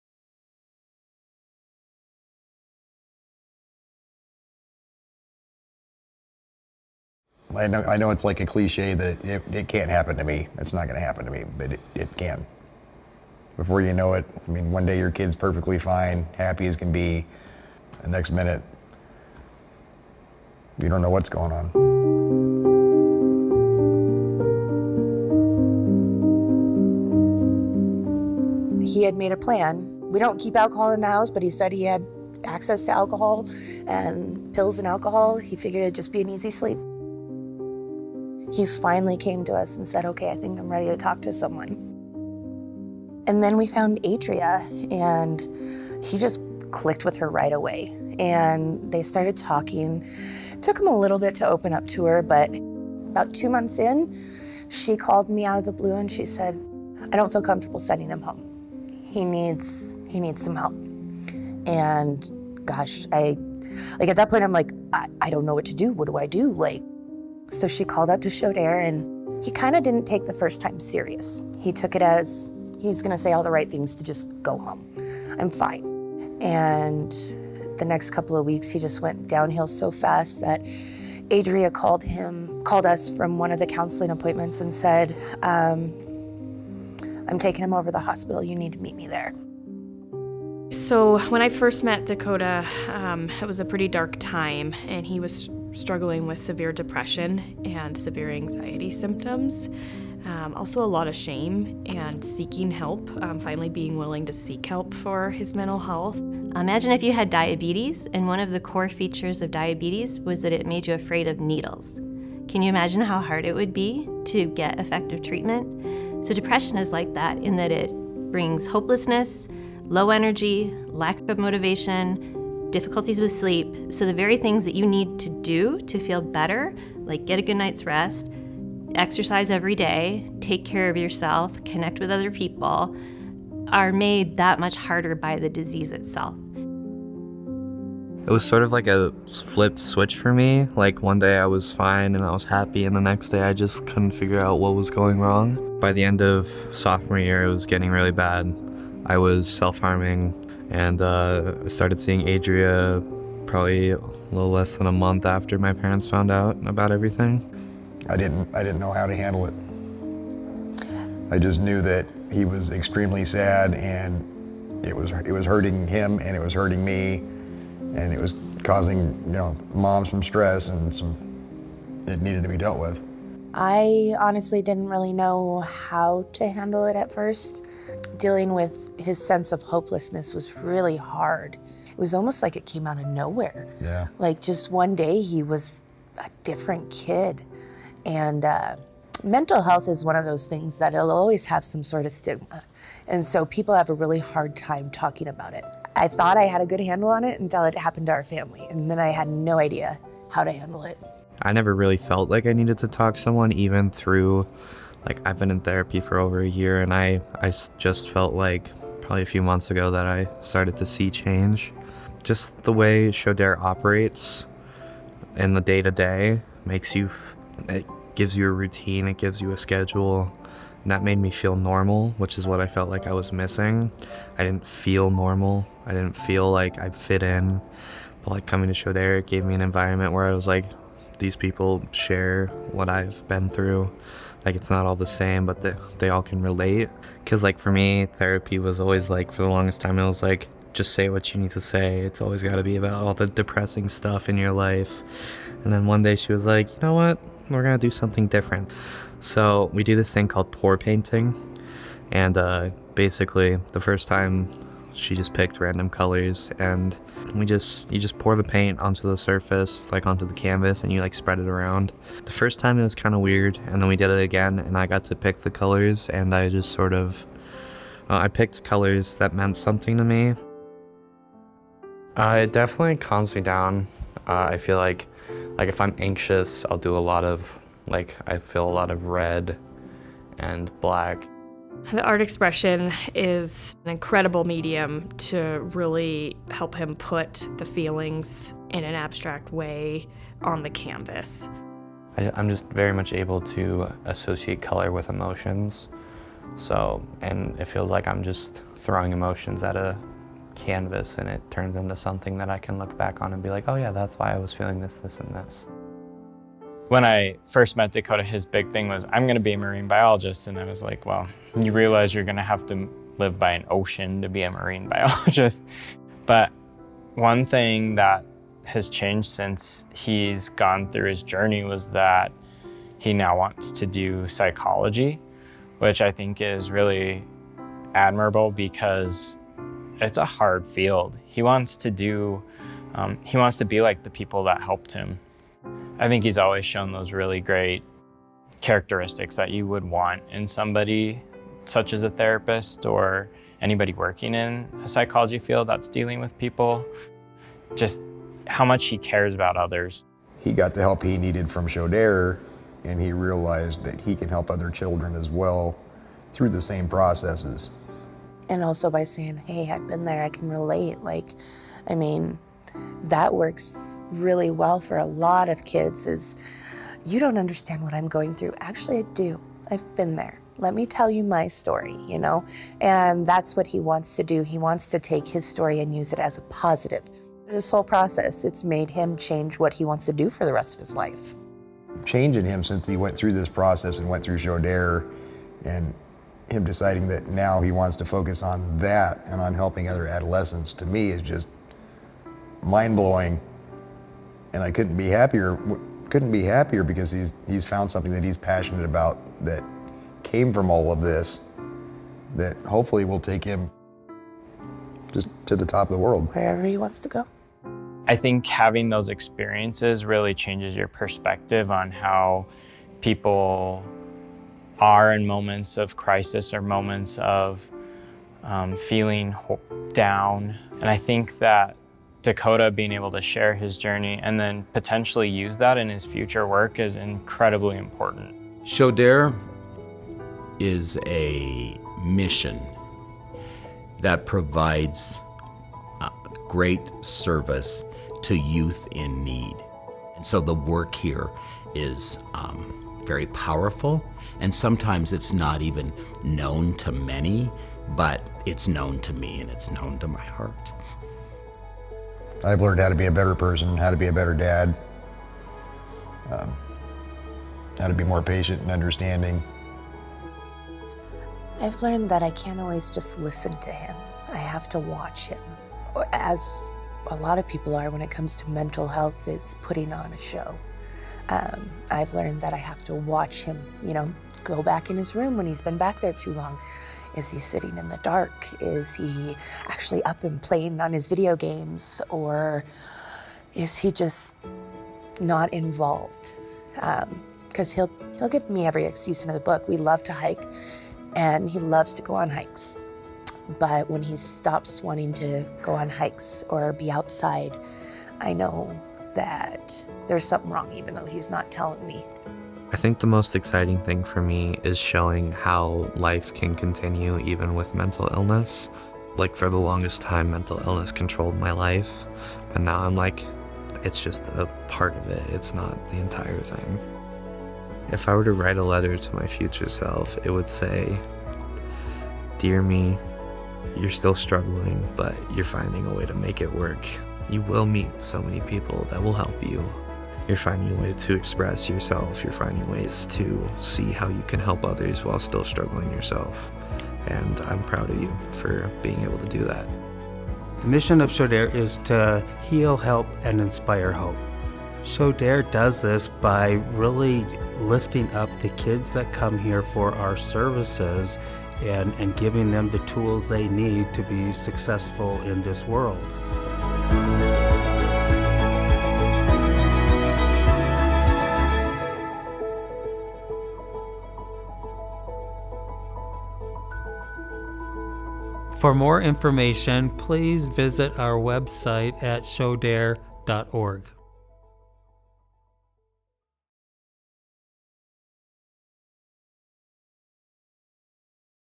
2022 CMN Dakotah Hurley
Dakotah Hurley is named 2022 Champion for Montana's Children's Miracle Network Hospital. Hurley, a former patient at Shodair, shares his experience of learning to manage his mental health.